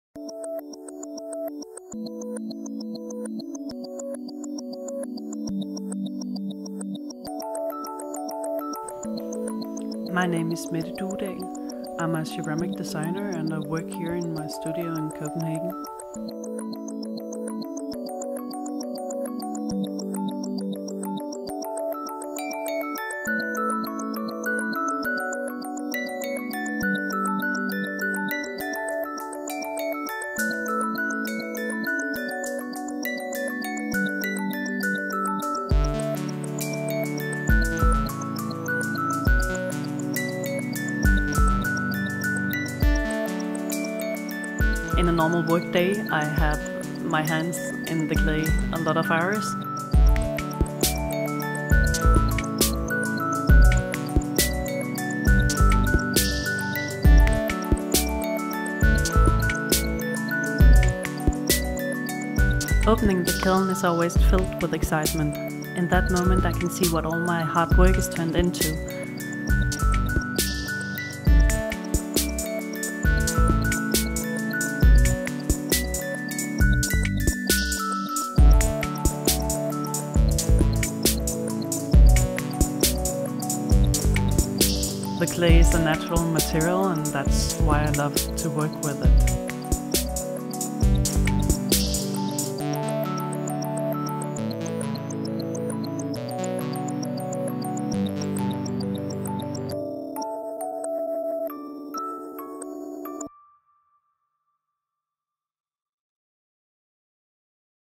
Muuto Studio visit at Mette Duedahl
Op bezoek bij Mette Duedahl, de ontwerper van de Push Coffee Maker voor Muuto.